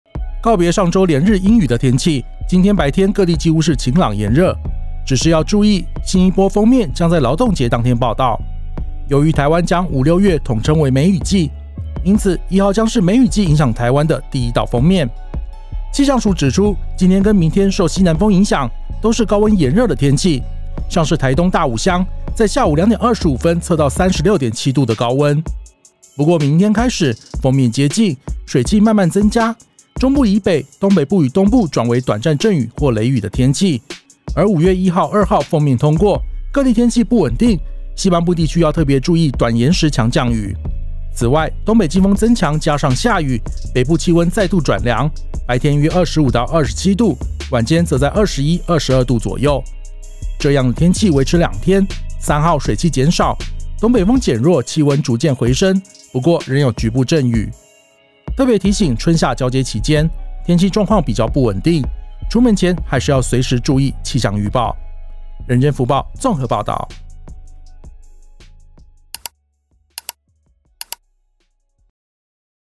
梅雨季要來了！勞動節首波鋒面報到
梅雨來了，中央氣象署指出，周三、四有一波鋒面影響台灣，台灣將5、6月統稱梅雨季，周三、四剛好5月1日及2日，是梅雨季影響台灣的第一道鋒面。